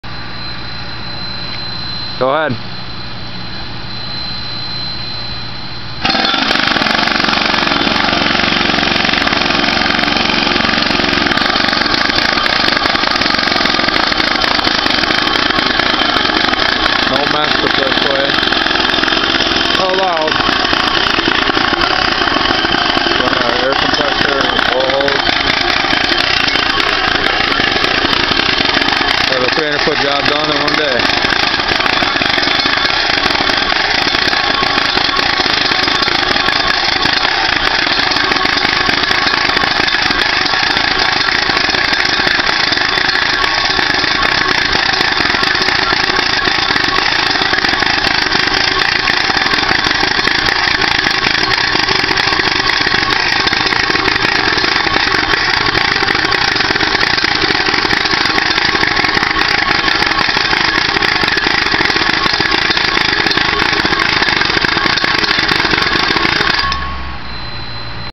Driving Fence Post Video #1 (We drive our fence posts 42" below grade)
What are the benefits of driving chain link posts then using typical concrete?

1. Quick installation we can usually finish any residential chain link fence in 1 day start to finish. (most companies have a set crew day and a finishing crew another day)

2. Cleaner installations we don't have to dig holes in your yard, no large equipment or wheelbarrow's making tracks in your yard.

3. Also digging holes in your yard can be a headache if you have a sprinkler system chances of hitting lines are increased. (We are only driving between a 2.5" - 1 5/8" post in diameter)

5. Wet back yards posts cemented can become loose and unleveled because of the ground around it causing structural damage. (Driven posts are not affected because they is no movement even if top soil is loose)

4. But the most important to us is post heaving! With are drastic weather changes cemented fence posts love to get pushed up from the frost which can lead to your fence being damaged. (Driven posts will not heave.)

See all the freeway fences around town those are driven!

Northland Fence